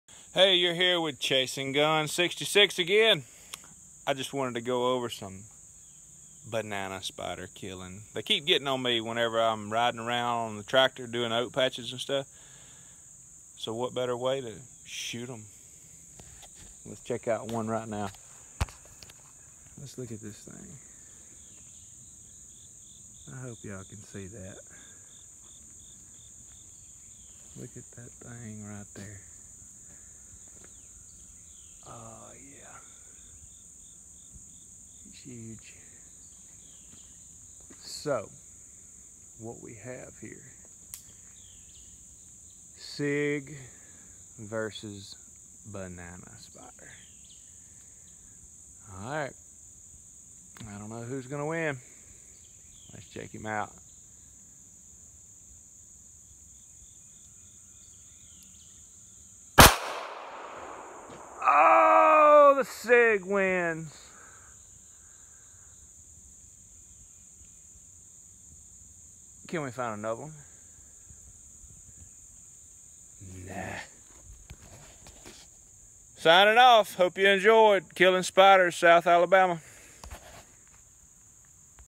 Did the spider live through getting shot
Shooting banana spiders , This video consist of spiders being shot with a pistol, sig p238 shooting banana spiders, guns shooting on hunting club, spider guts everywhere. This is what happens when a gun is carried to the club and spiders line the roads. I call it spider eradication.     Enjoy and God Bless